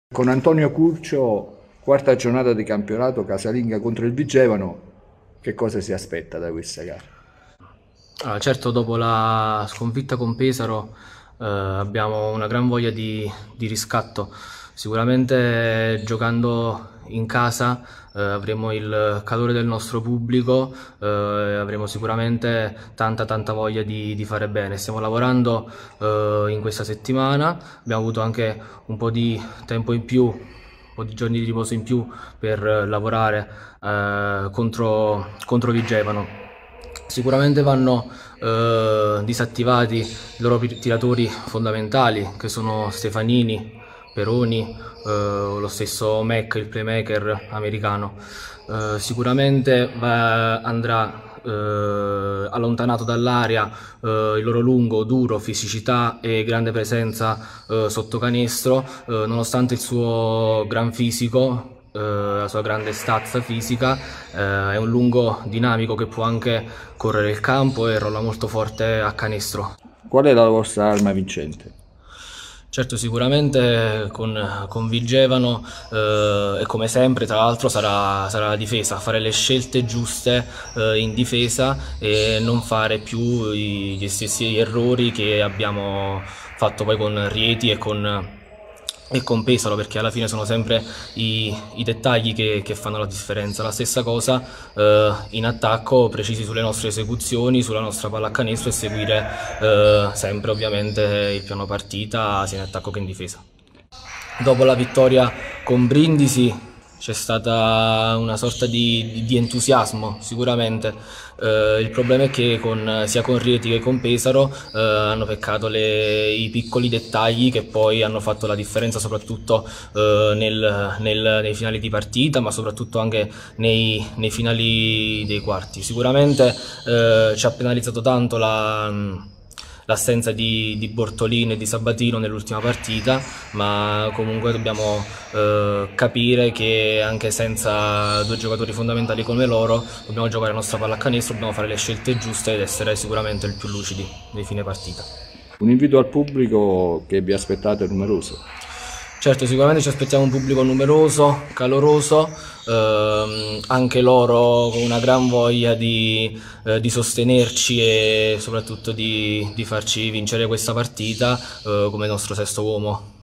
Curcio: "I piccoli dettagli faranno la differenza contro Vigevano"
L'assistant coach dell'Avellino Basket, Antonio Curcio, presenta il match contro Vigevano.